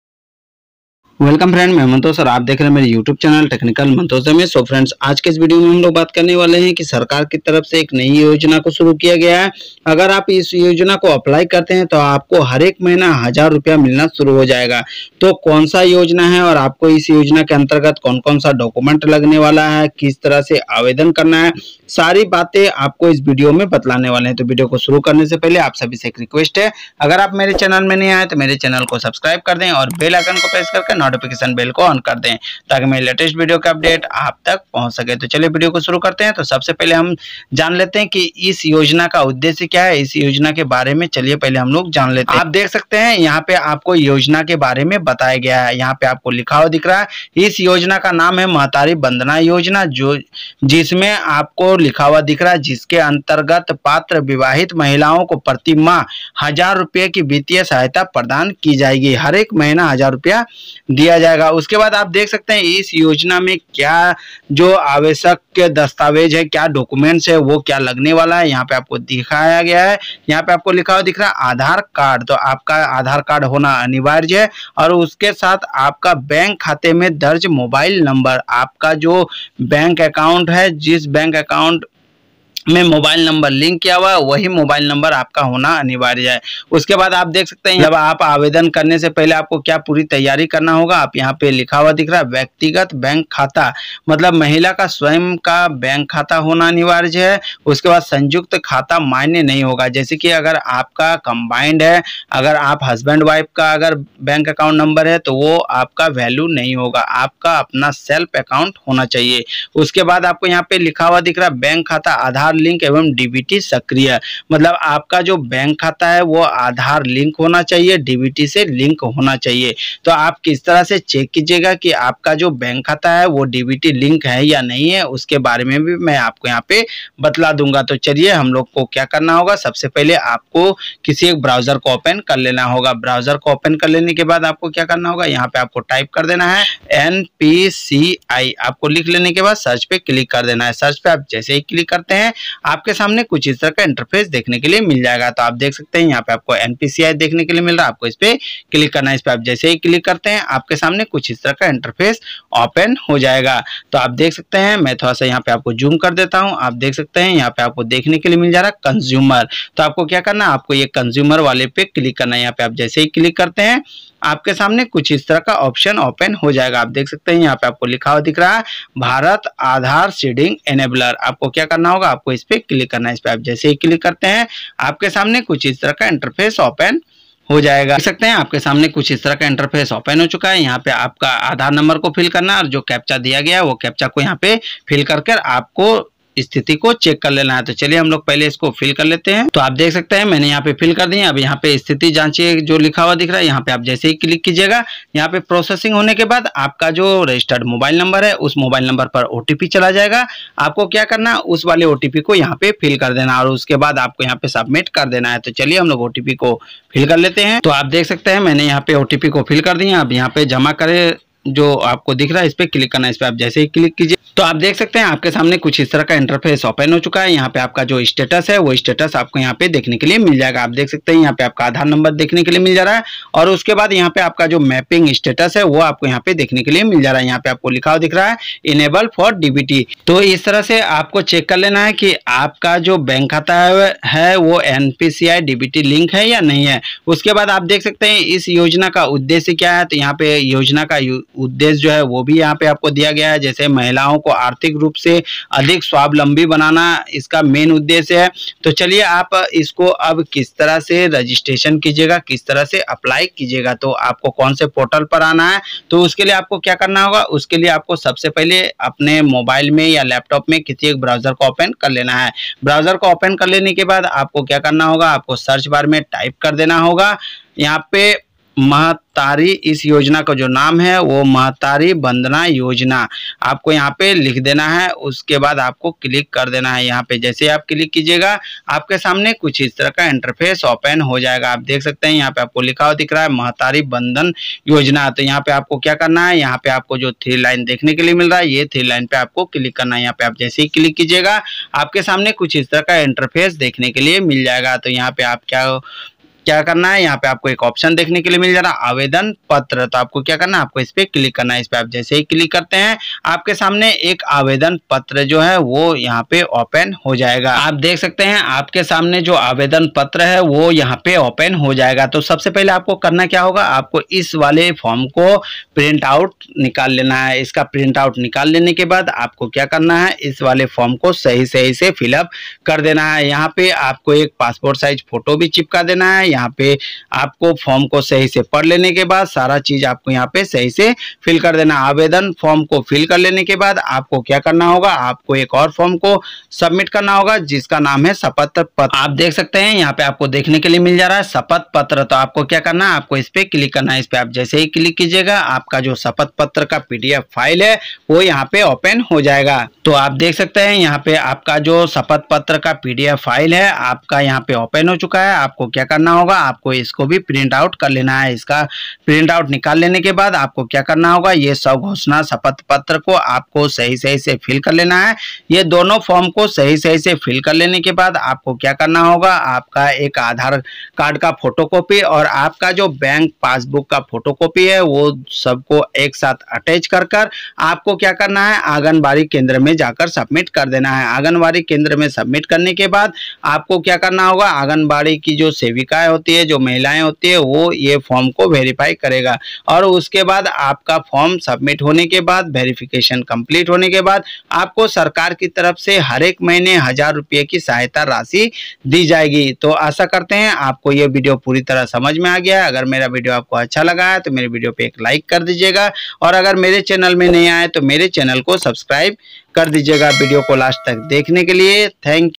Mahtari Vandana Yojna form kaise bhare | New Government MVY Scheme Apply 2024, Milega ₹1000/- Mahina
Friends aaj ke is video me bataya gaya hai ki sarkar ki taraf se ek nayi scheme ko suru kiya gaya hai, Agar app is scheme ko apply karte hai to sarkar ki taraf se harek mahina 1000/- milega.